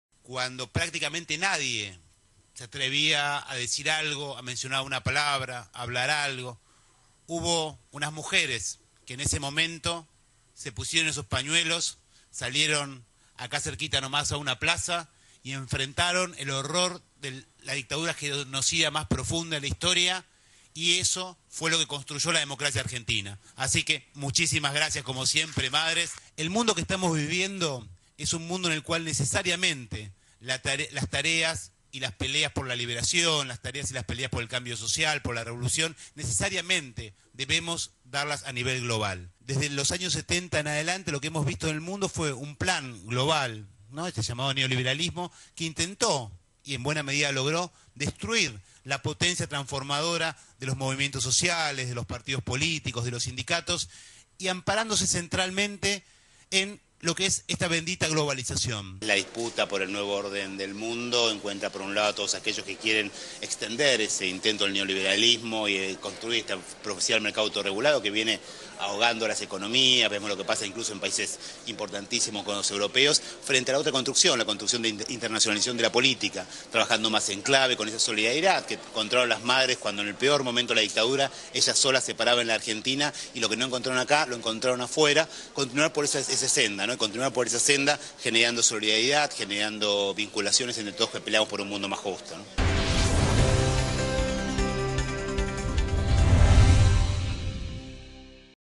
Abal Medina participó del Congreso Internacional de Salud Mental y Derechos Humanos
El jefe de Gabinete Juan Manuel Abal Medina participó del XII Congreso Internacional de Salud Mental  y Derechos Humanos organizado por la Asociación Madres de Plaza de Mayo.
Durante el encuentro, Abal Medina agradeció a las Madres por "la lucha que dieron en el momento más difícil de la historia argentina, enfrentando a la dictadura más atroz, cuando nadie se atrevía a decir nada".
"Gracias a esa lucha recuperamos la democracia", destacó el funcionario.
Por otra parte, Abal Medina enfatizó que "el neoliberalismo buscó la destrucción de los movimientos sociales utilizando como instrumento principal la globalización, por eso es tan importante trabajar con los movimientos sociales a una escala internacional y la unión regional, no basada en el comercio sino en la política, tal como la Unasur".
En este sentido, el jefe de Gabinete aseguró que "las construcciones desde lo político son las que valen la pena porque son las que pueden transformar la realidad".
"Es necesario seguir dando esta batalla, no hay que renunciar al tablero global, porque esa es la garantía de que vamos a construir un mundo más justo", concluyó.
El XII Congreso Internacional de Salud Mental y Derechos Humanos "El otro soy yo" comenzó ayer en la Ciudad de Buenos Aires con el eje puesto en lograr construcción colectiva para elaborar un Plan Nacional de Salud Mental y en la desmanicomialización de los pacientes.
La actividad, que cuenta con la presencia de especialistas de todo el mundo, se desarrollará hasta mañana en el auditorio de la Universidad de las Madres de Plaza de Mayo.